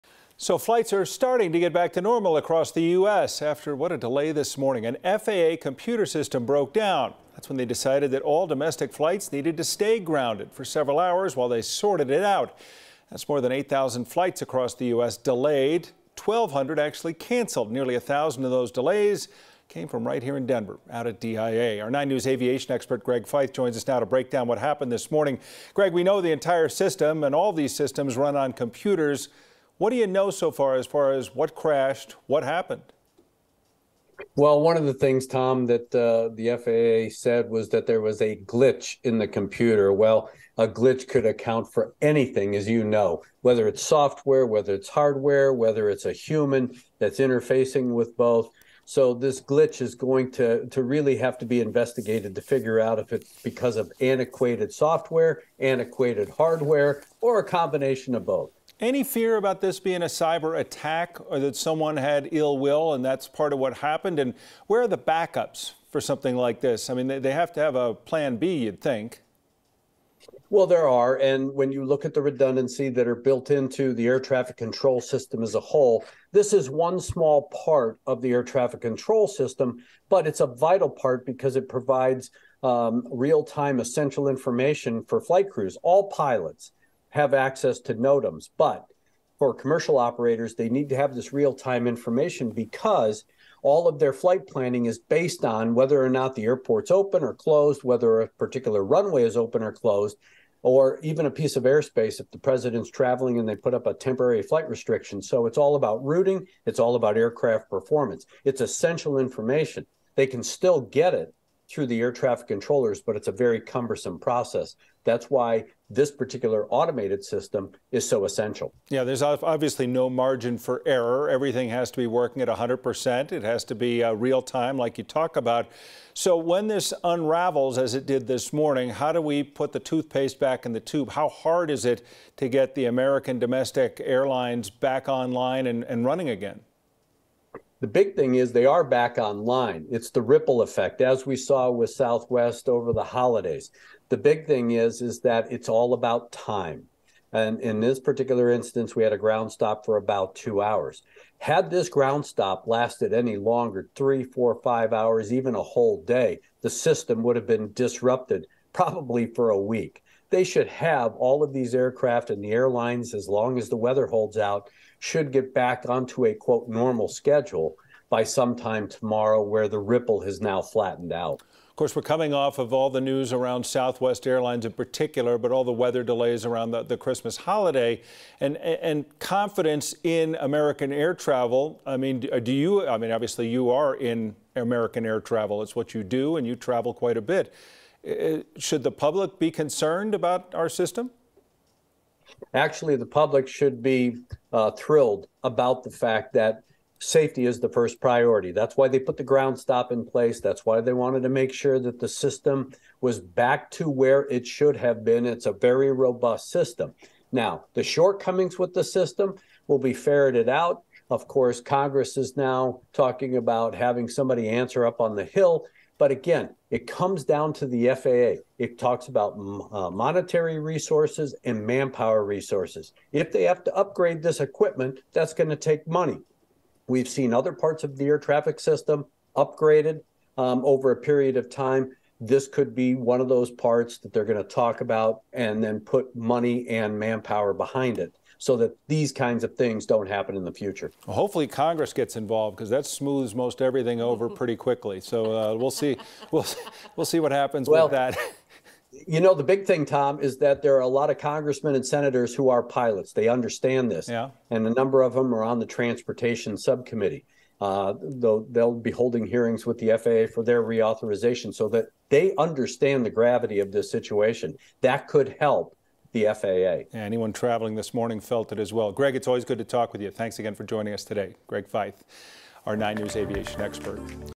FAA Outage Update: Damaged Database Caused System To Fail
Flights are slowly getting back to normal after an FAA computer system broke down forcing all planes in the U.S. to stay grounded for several hours.

9NEWS Aviation Expert Greg Feith joins us now to break down what happened this Wednesday morning.

9NEWS (KUSA) is located in Denver, Colorado.